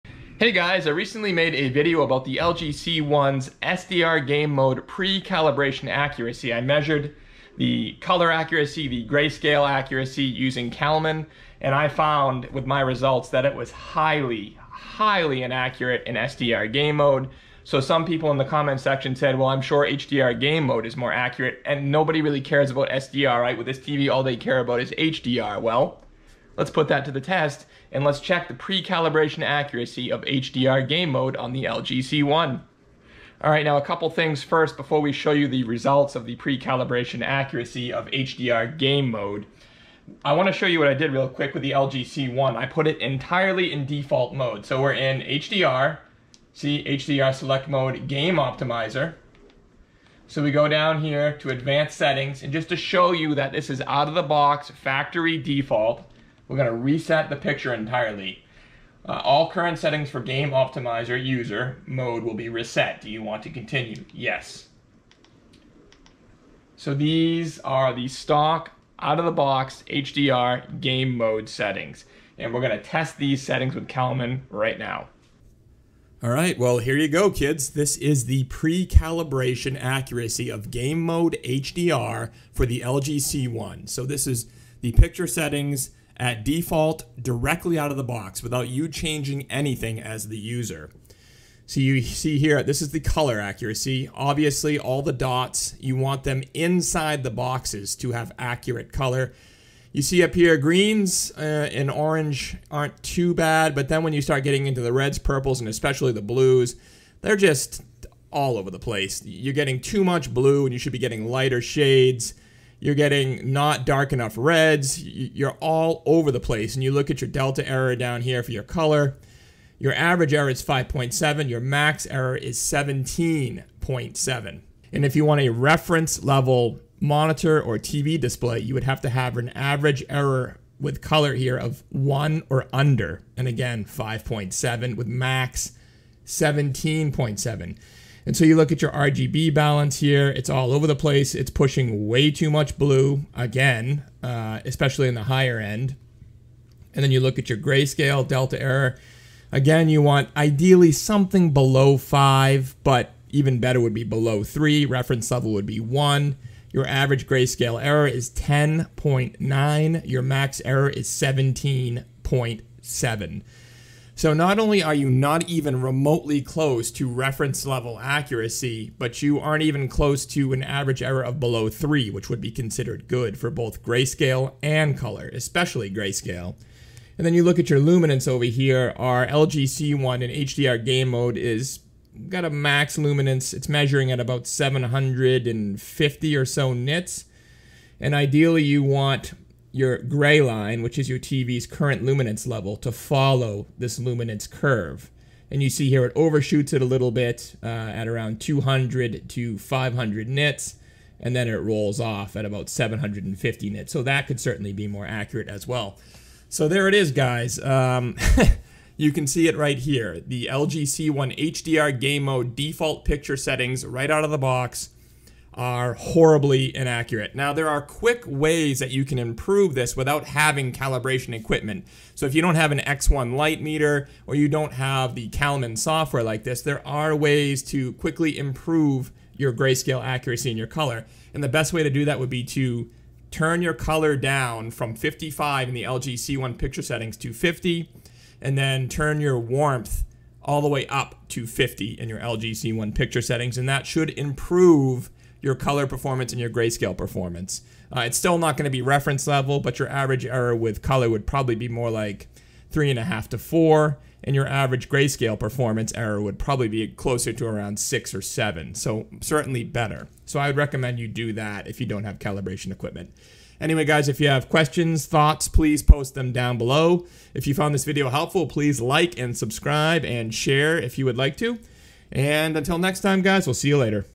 LG C1: HDR game mode pre-calibration accuracy is...well...
In this video, we analyze the grayscale, color, and luminance accuracy of the LG C1 in HDR game mode with default settings out  of the box.  Hint: its bad, BUT, you can do something about it, even without calibration equipment.

Timestamps:
Intro - LG C1 set to factory default HDR game mode settings: 00:00
Pre-calibration Accuracy results for HDR game mode: 1:15
How to improve HDR game mode accuracy without calibration gear: 3:36